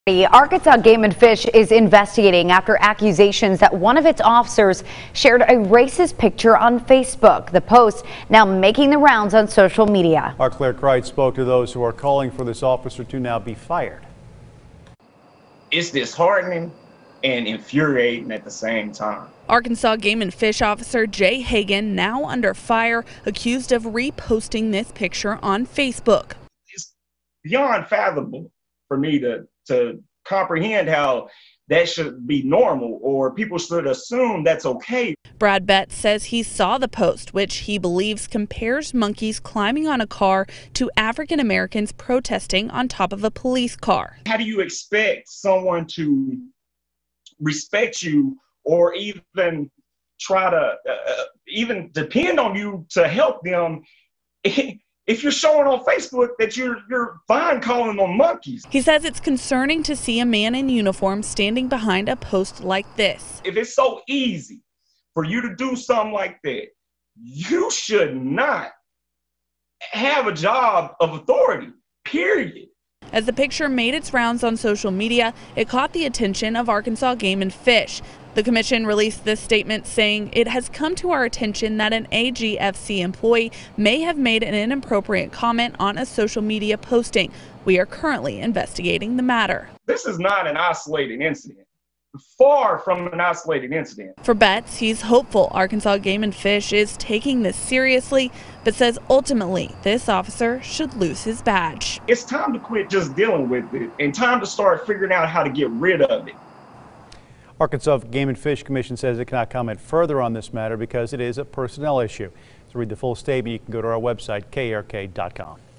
Arkansas Fish and Game investigating officer's post critics call racist
The Arkansas Game and Fish Commission is investigating accusations that one of its officers shared a racist picture on Facebook.